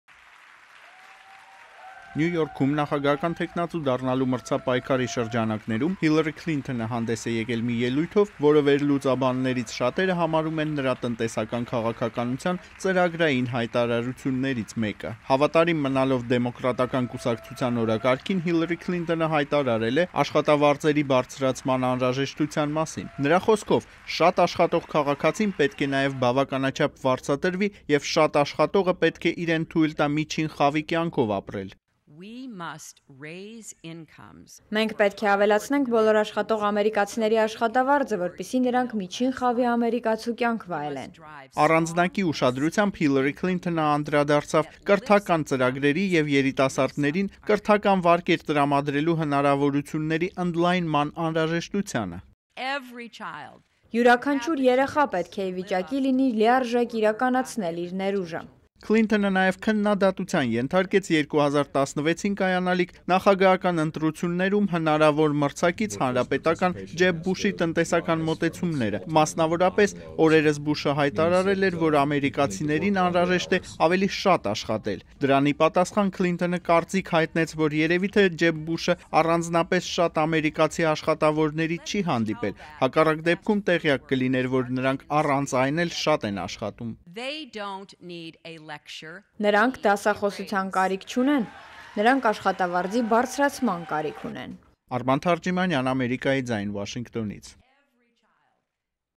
Քլինտոն. Շատ աշխատողը պետք է լավ ապրի
Բոլոր նրանք, ովքեր շատ են աշխատում, արժանի են վարձատրվել այնպես, որպեսզի միջին խավի կյանք վայելեն, համոզված է դեմոկրատ Հիլարի Քլինթընը: